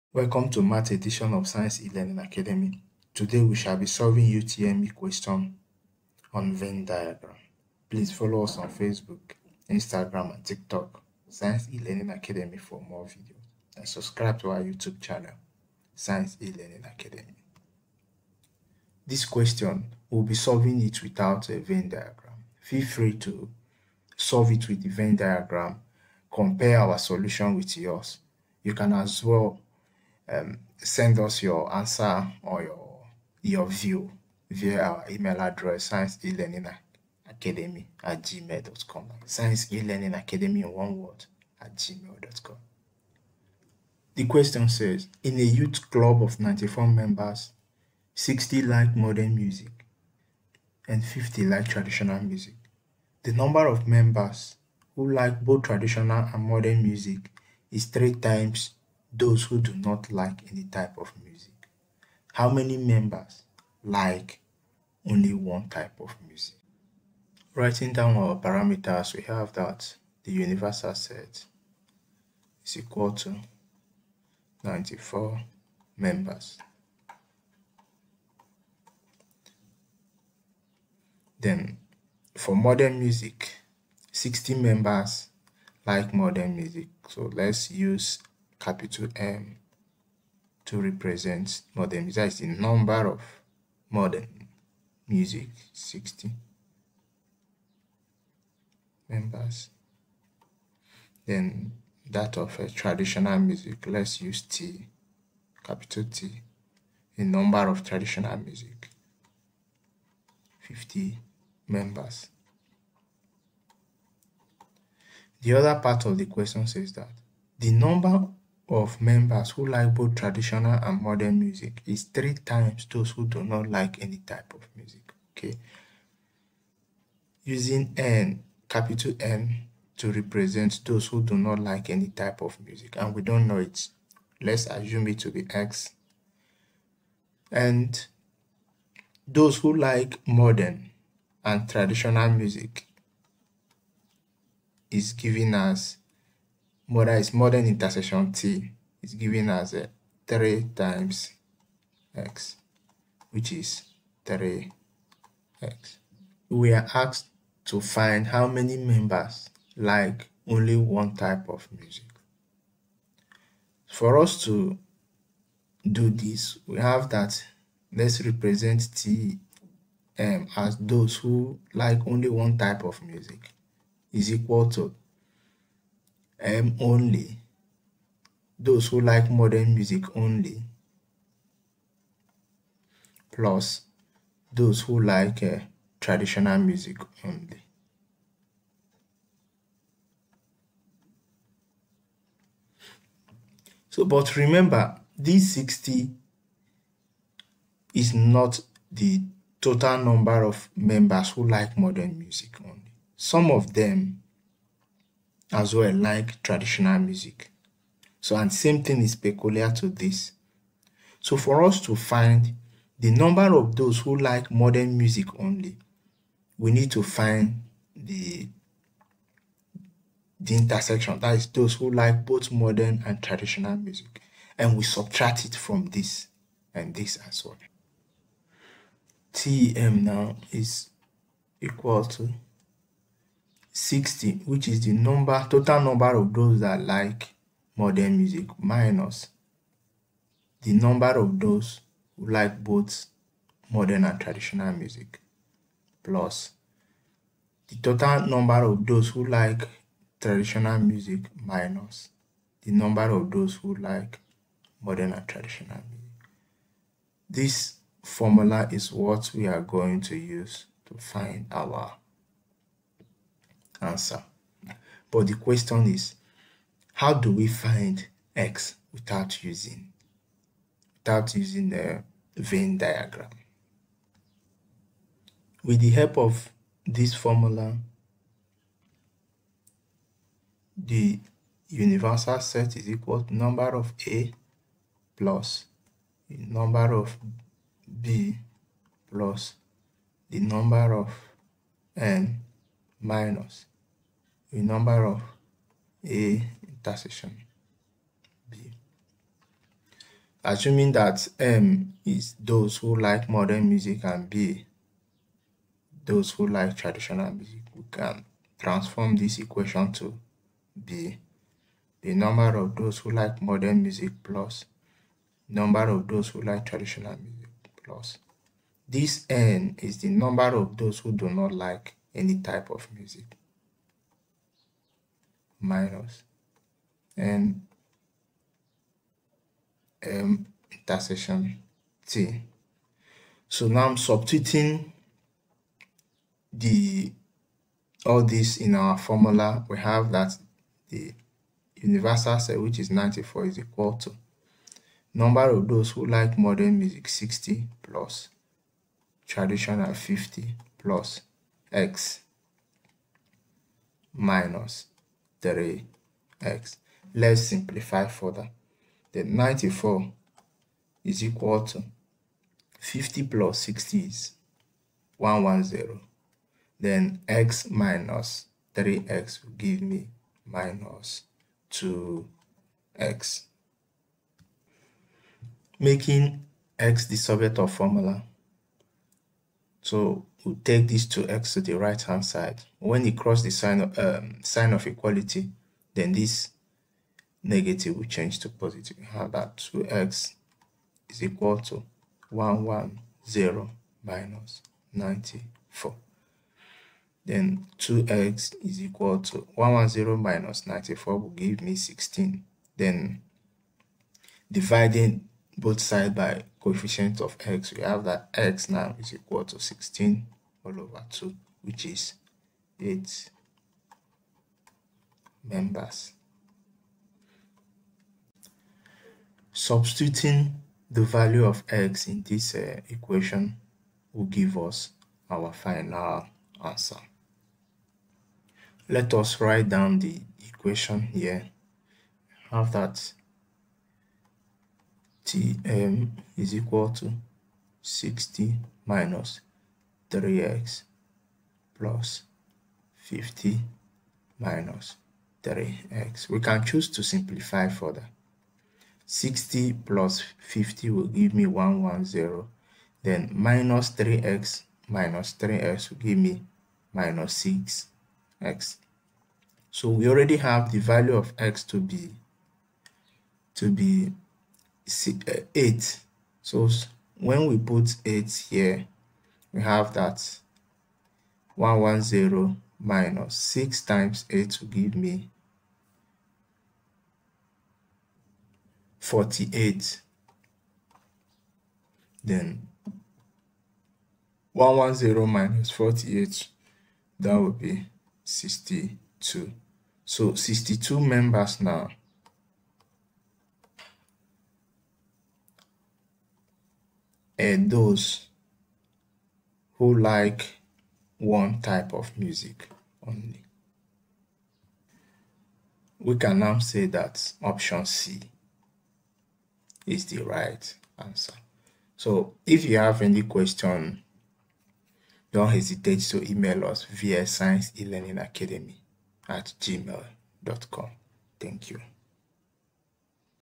Solution to UTME question on Venn diagram
This video explained a simple approach to solve questions pertaining Venn diagram with a formula.